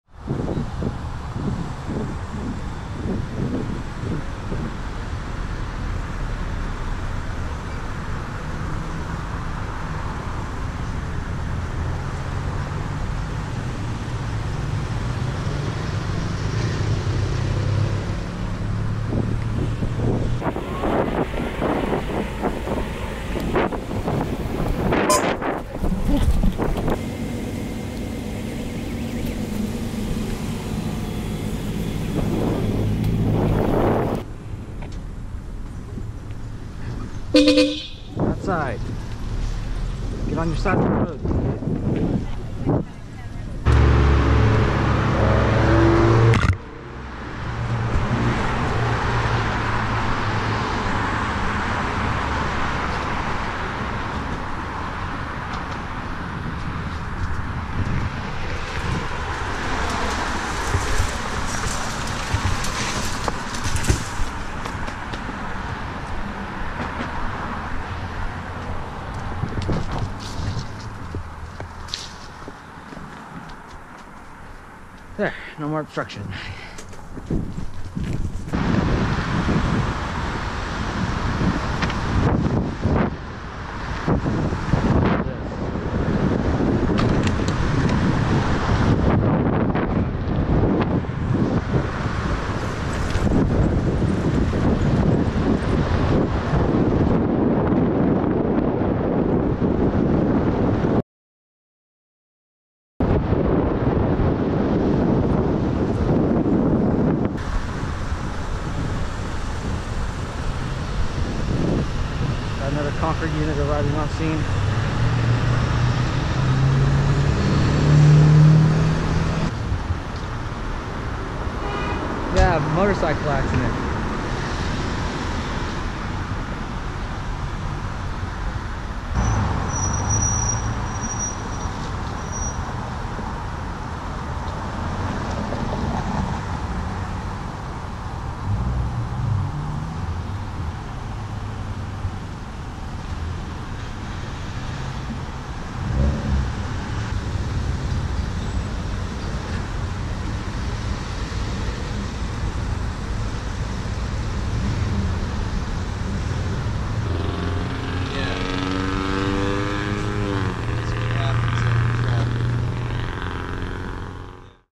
Bike Lane obstructions, Wrong Way Driver + Moto Crash
The first clips are from the same spot. This is a routine place where people block intersection.  The next clip is in a parking lot where the driver did not drive on his side If the road. He cut the corner and I had to honk at him to move over.  It rained pretty hard and was windy the past few days, so a tree branch fell in the road. I safely moved it out of the bike lane so others will not have to ride in traffic to pass.  The accident involving a motorcycle rider was not his fault. He was in the turning lane and a driver cut him off, making him fall off his bike. He was not injured but his bike suffered some minor damage.